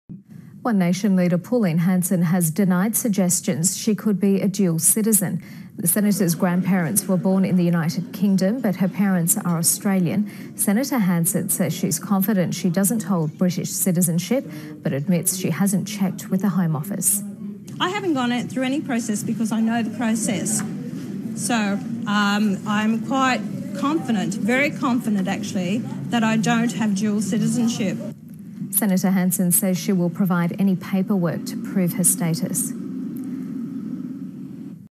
Pauline Hanson's grandparents were born in the UK but her parents were born in Australia
7/11/2017 "I haven't gone out through any process because I know the process so, um, I'm quite confident, very confident actually that I don't have dual citizenship," Pauline Hanson says.